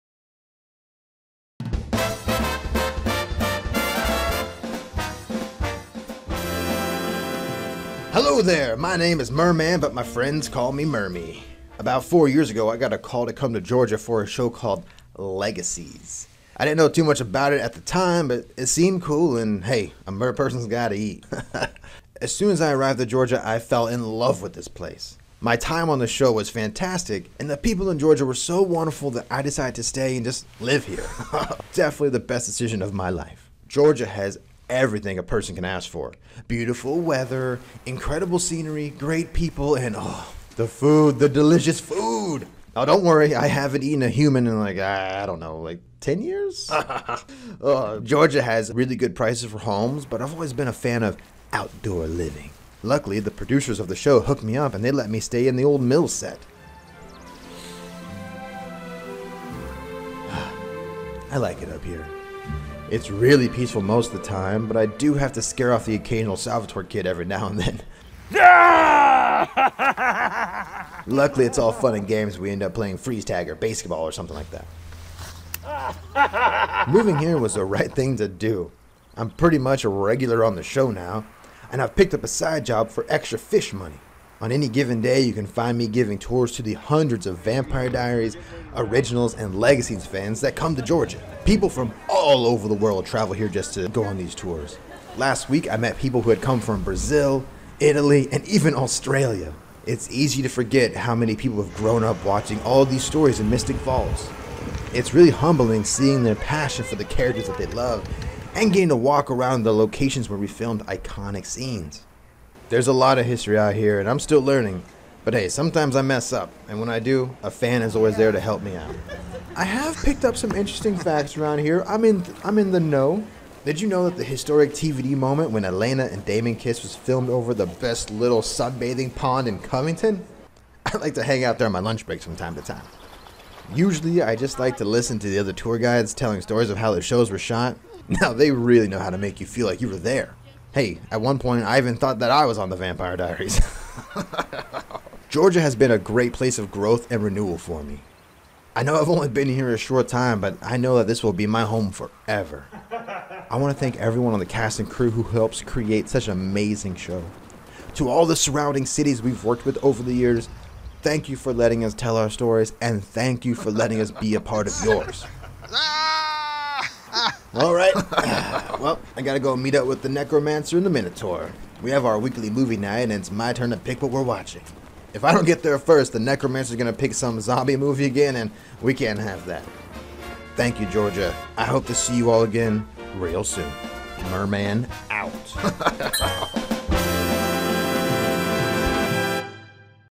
Merman on "Legacies," and Deciding to Stay in Georgia
That's right - it's Merman leading tours in Covington, taking in the sights around Georgia and planning his movie night with Necromancer. Thank you "Legacies," "The Originals," and "Vampire Diaries" for this amazing Merperson, and for the years of film tourism Covington will continue to enjoy. Au revoir!